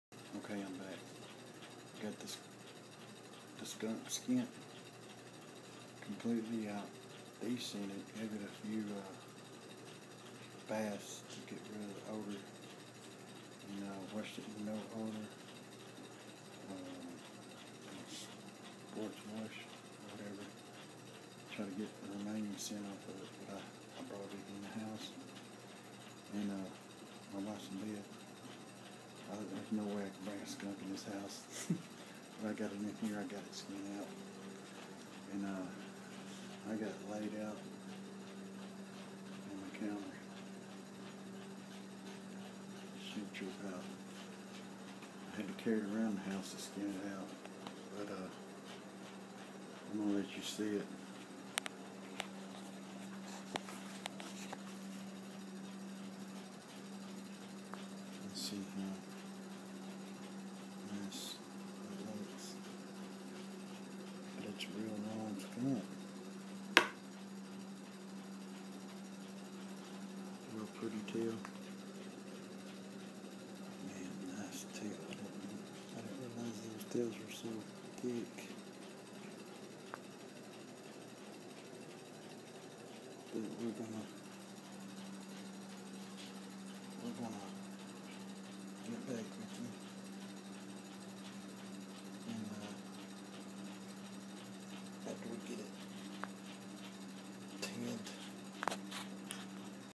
Taming the skunk hide
First time trying a skunk but I skinned it but removed the sent bags first but cam out great it was road kill  I picked up on way to my job will make a hat out of it yea